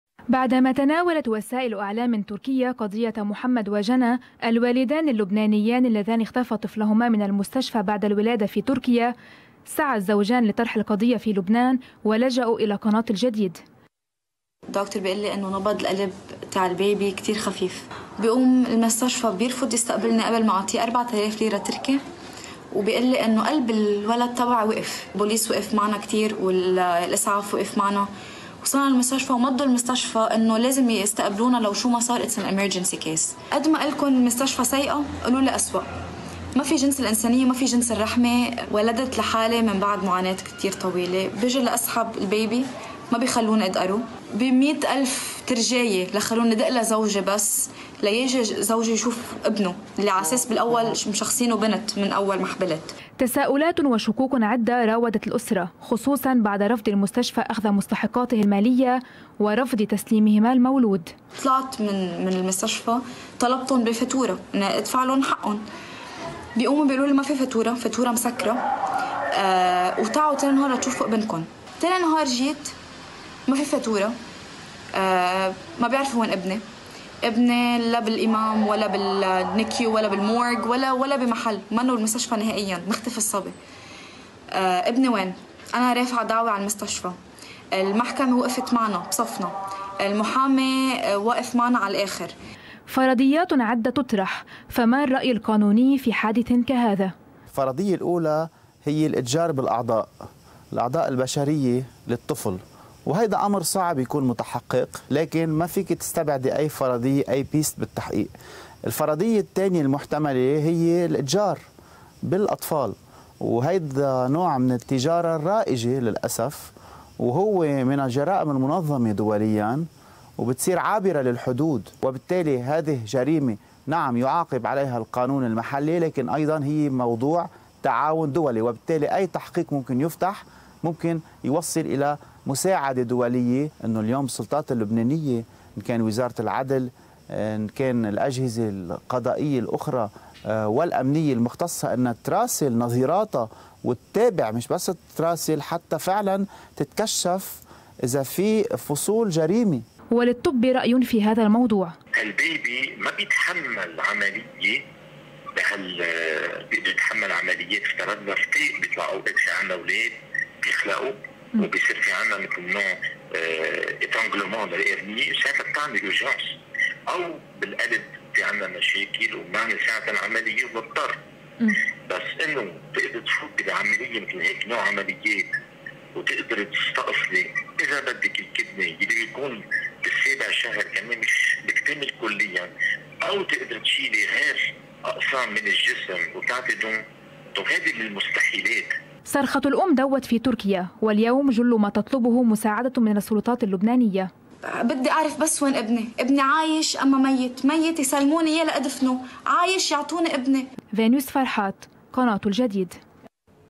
سيدة لبنانية تلد في تركيا ويختفي مولودها من المستشفى
بعدما تناولت وسائل اعلام تركيا قضية محمد وجنى الوالدان اللبنانيان الذان اختفى طفلهما من المستشفى بعد الولادة في تركيا سعى الزوجان لطرح القضية في لبنان ولجأوا الى قناة الجديد
تساؤلات وشكوك عدة راودت الأسرة خصوصا بعد رفض المستشفى أخذ مستحقاته المالية ورفض تسليمهما المولود
فرضيات عدة تطرح  فما الرأي القانوني في حادث كهذا؟